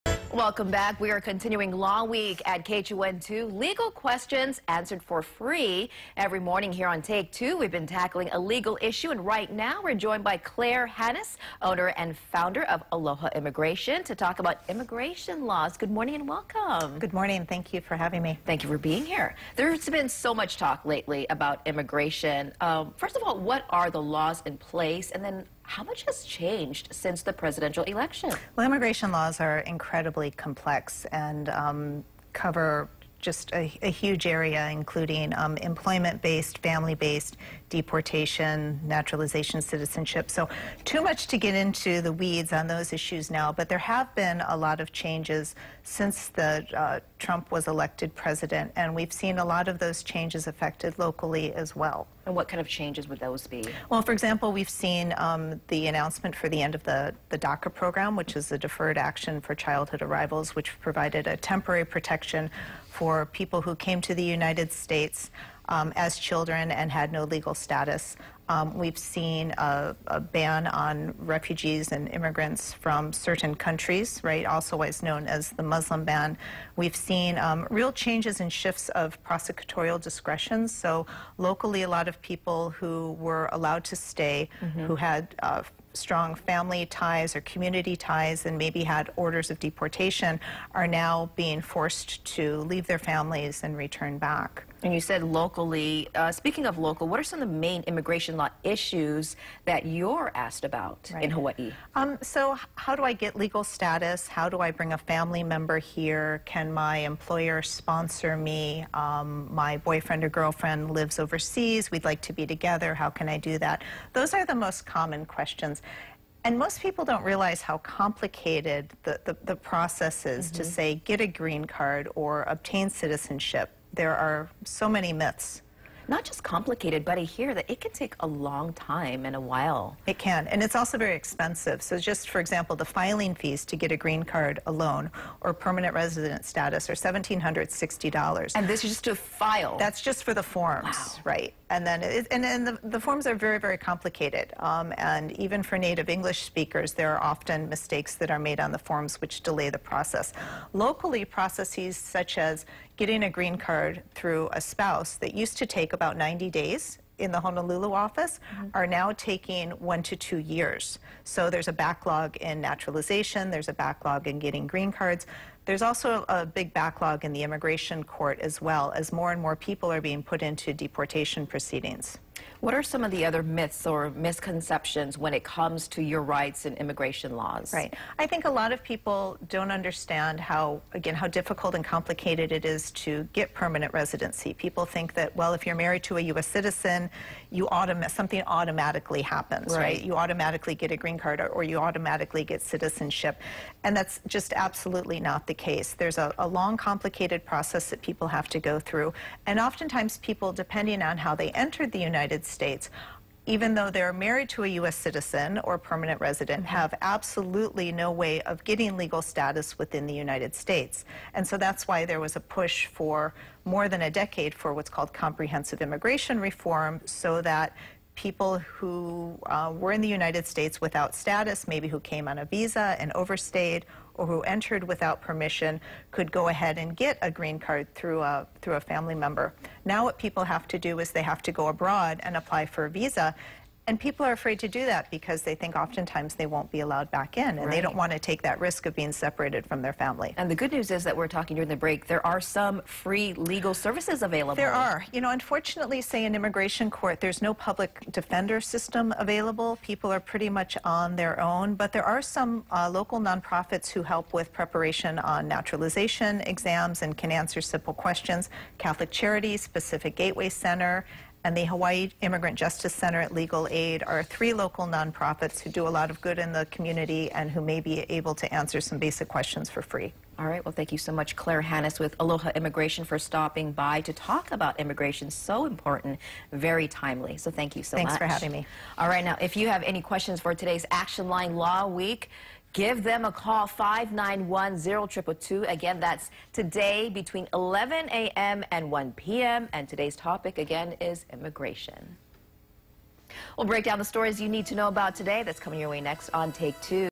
Law Week: Immigration and Your Legal Rights
It's an issue that's been making headlines lately. Immigration has been a hot topic, but a lot has changed since the presidential election. What are your legal rights? Clare Hanusz, owner and founder of Aloha Immigration, joined Take2 to talk about it.